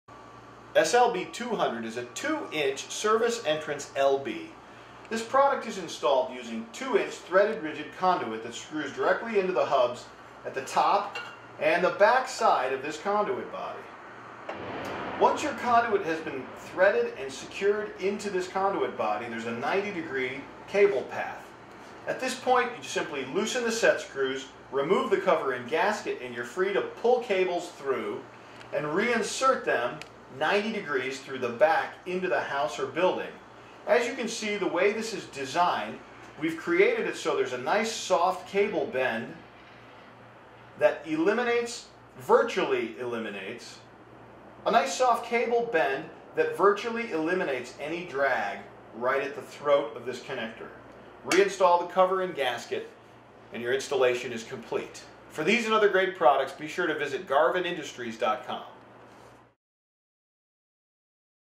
Conduit Bodies: 2" Aluminum LB Style Service Entrance Conduit Body SLB200
- 2" LB style service entrance conduit bodies are attached to 2" threaded rigid or IMC pipes in opposite directions. Their removable cover makes general maintenance and wire pulling and feed through easy and convenient. Commonly used for electrical service entrance wires or as an economical convenience box when extra wire capacity is needed. Their extra wire capacity and angled body allow installer to easily bend wires into the smooth turns. They have a powder-coated finish and a water-tight gasket to ensure a weatherproof installation.